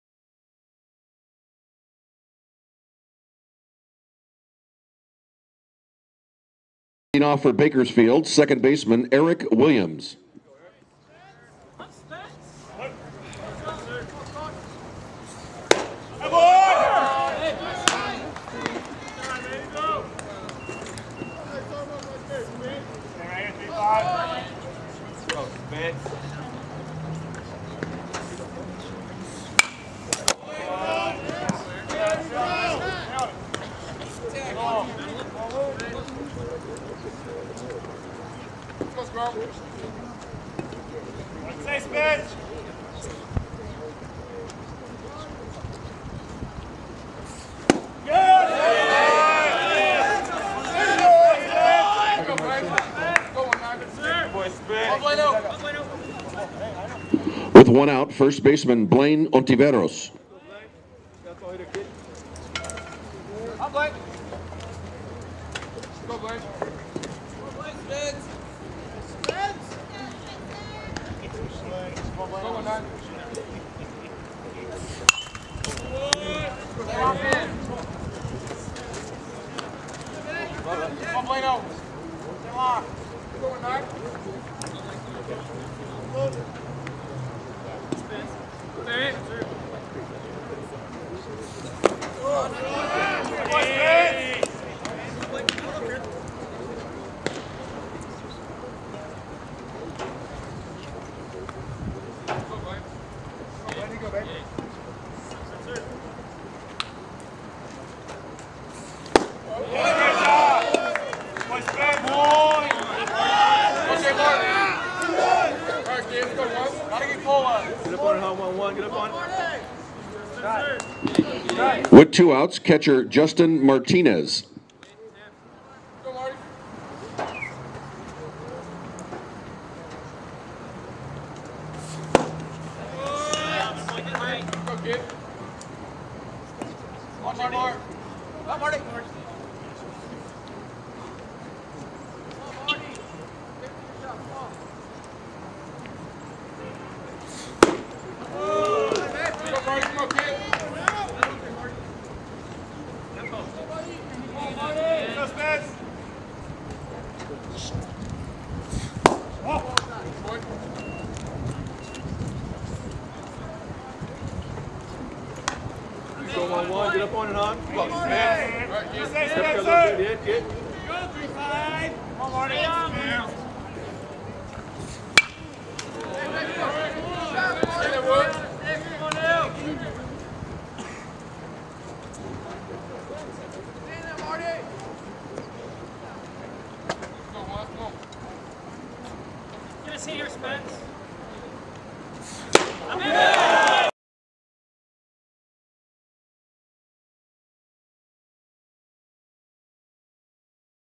Spencer Jack v Bakersfield 3.7.2013
Spencer Jack committed to the University of Kentucky.

GLENDALE COLLEGE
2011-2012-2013 WESTERN STATE CONFERENCE SOUTH CHAMPIONS